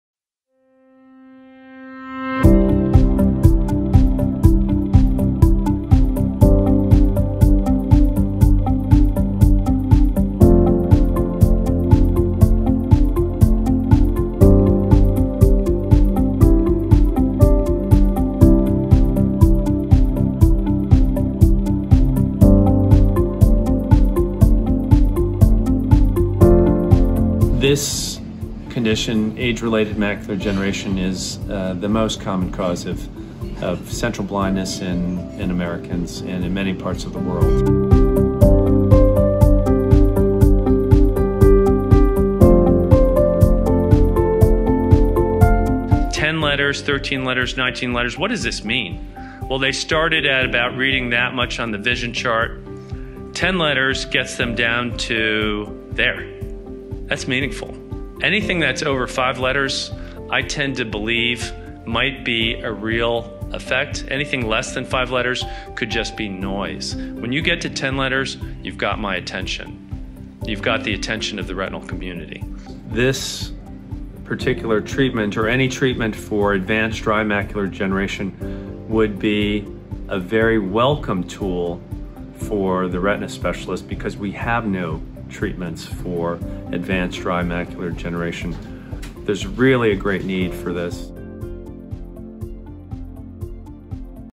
Dr. Allen Ho - Why is dry age-related macular degeneration (dry AMD) research so important?
Allen C. Ho, M.D., Wills Eye Hospital Director of Retina Research, on why dry age-related macular degeneration (dry AMD) research is so important and OpRegen®, $LCTX's retinal pigment epithelium (RPE) transplant therapy for dryAMD.